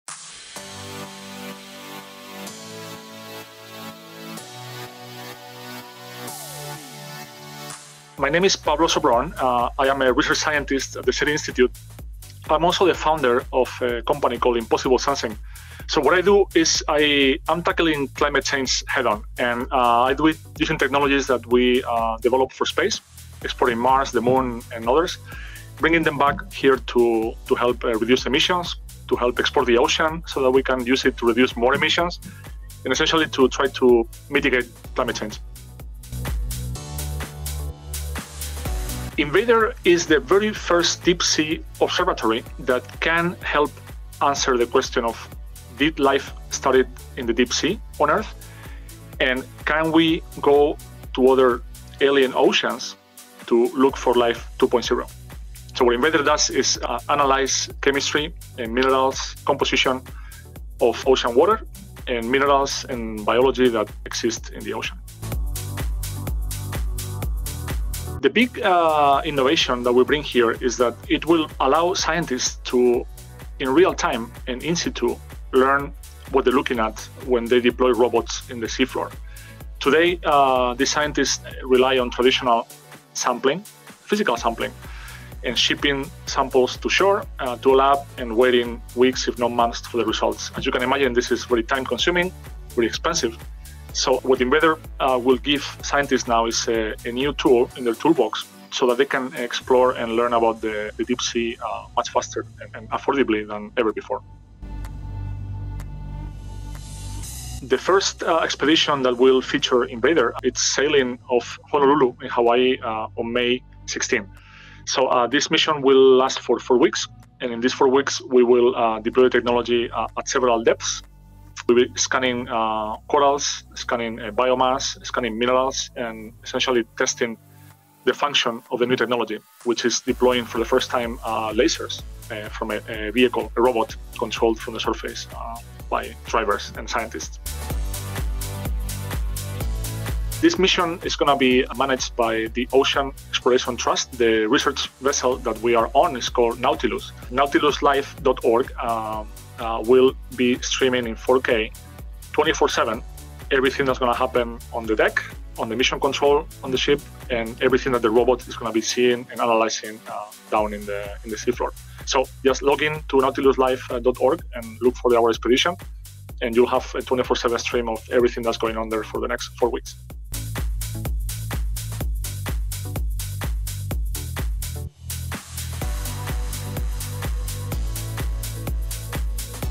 InVADER Mission Conducts Deep-Sea Test, ft. Dr. Pablo Sobron
We asked project lead Dr. Pablo Sabron three questions about the InVADER (In-situ Vent Analysis Divebot for Exobiology Research) Mission, which aims to advance technologies to explore, characterize and sample the seabed here on Earth.